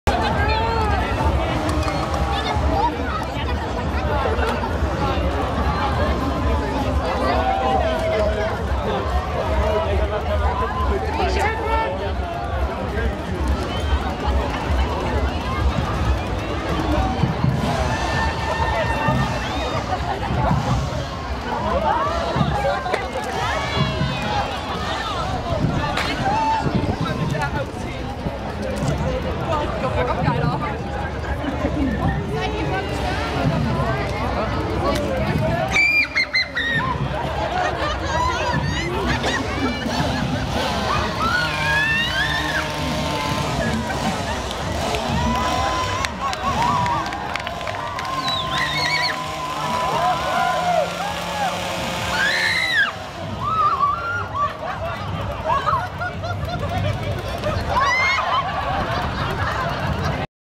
Zombie walk Sommerumzug Neuhausen NZA Althengstett 2012-6
Sommerumzug in Neuhausen im Enzkreis.
Die Narrenzunft Althengstett läuft natürlich im Sommer nicht im Häs...wir waren als Zombies dabei.